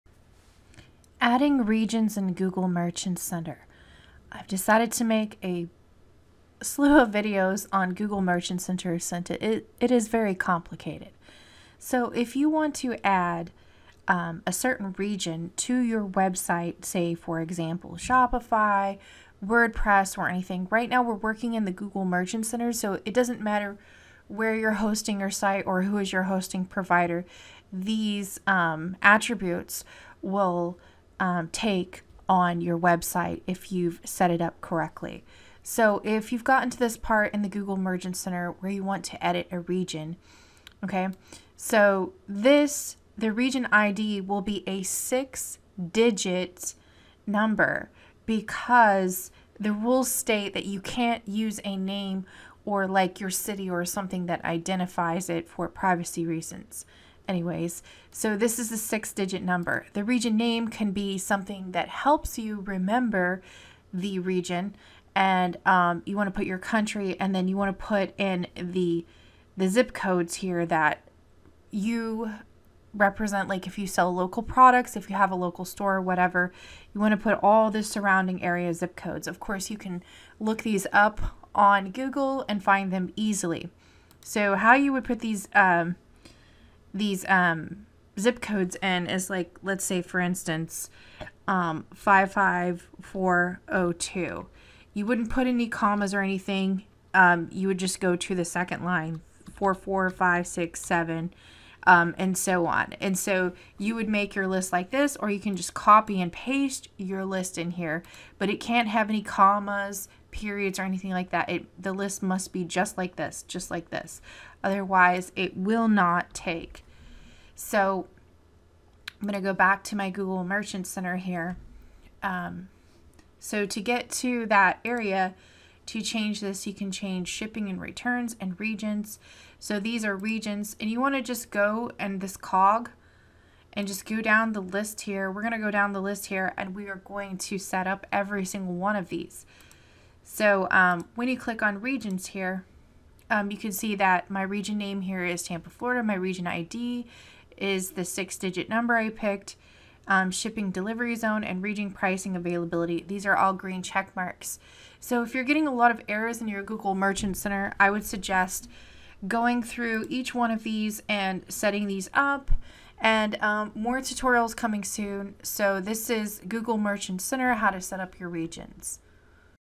Google Merchant Center Tutorials -How to set up Regions
In this video we learn how to set up regions in your local area for listings on the google merchant center.